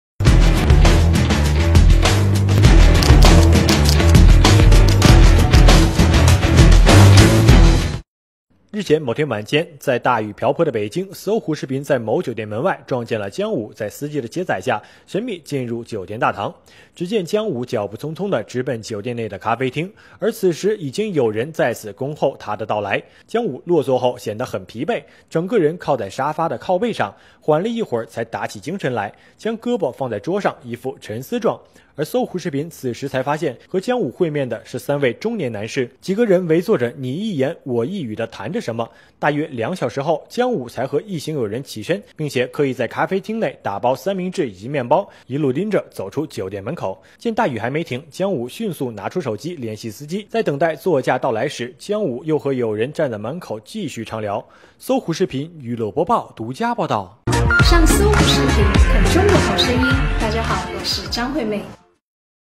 《搜狐娱乐》姜武深夜酒店会友 冒雨与友人们畅聊
【搜狐视频娱乐播报】报道 日前某天晚间，在大雨瓢泼的北京，搜狐视频在某酒店门外却撞见姜武在司机的接载下，神秘进入酒店大堂。 只见，姜武脚步匆匆地直奔酒店内的咖啡厅，而此时已经有人在此恭候着他的到来。姜武落座后便显得很疲惫，整个人靠在沙发靠背上，缓了一会后，才打起精神，将胳膊放在桌子上，一副沉思状。而搜狐视频此时才发现，与姜武"约会"的是三名中年男士，几个人围坐着你一言我一语地倾谈着什么。大约两个多小时后，姜武与一行友人起身，还刻意在咖啡厅内打包三明治及面包，一路拎着走出了酒店门口。 见大雨还未停，姜武迅速拿出手机联系司机前来接机，而在等待座驾到来时，姜武又与友人们站在酒店门外继续畅聊。